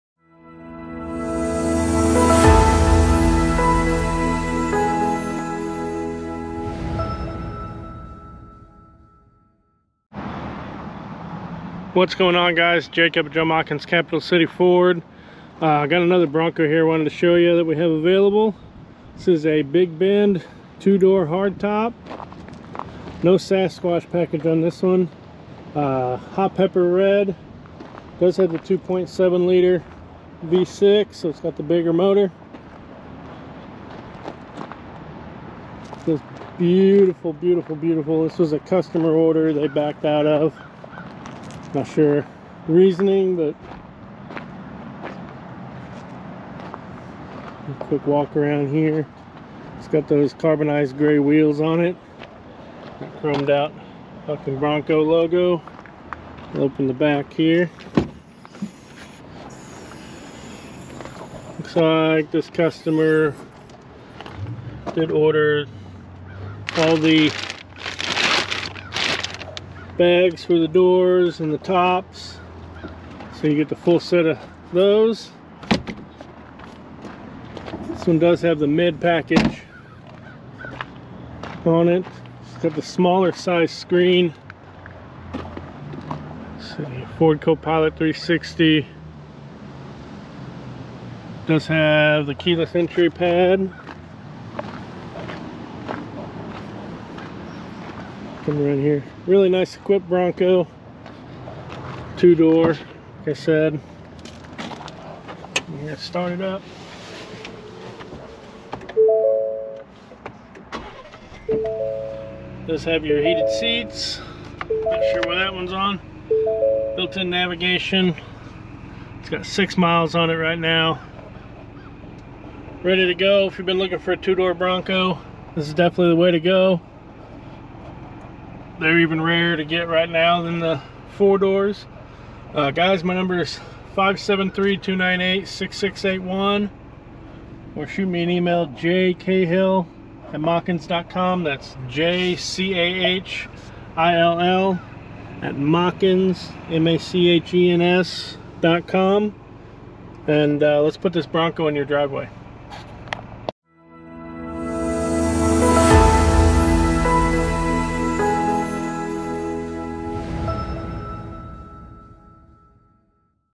2 Door Hot Pepper Red Ford Bronco!
Q: What has two doors and is cool as hell?
A: This stunning Big Bend Ford Bronco.

If you're interested in this or any other vehicles we have available be sure to reach out